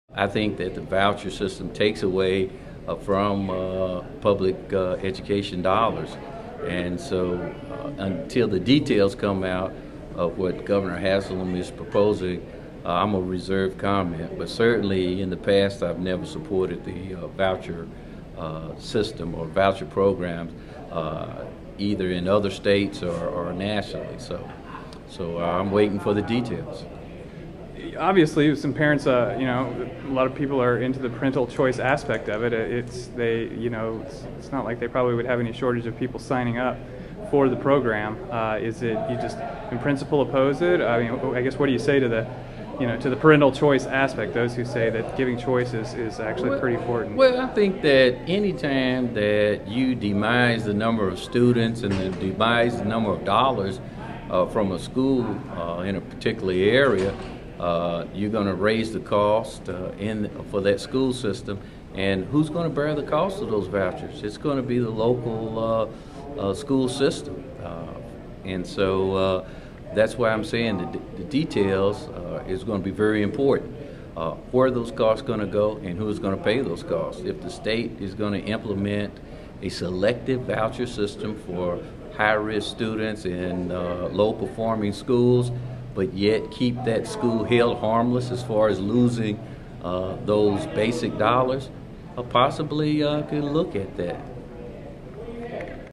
Armstrong Taking Wait-and-See on Voucher Bill (TNReport.com)
Rep. Joe Armstrong, a Knoxville Democrat, said he's generally opposed to voucher measures, but he's withholding judgement on legislation Bill Haslam said he supports in his state-of-the-state speech Monday night. Armstrong said it has been his position in the past that "anytime you demise the number of students and demise the number of dollars from a school in a particular area you are going to raise the cost for that particular school system -- and who is going to bear the cost of those vouchers?" Yet if the governor can devise a policy that will keep public schools "held harmless" from the loss of students, then Armstrong says he might support it.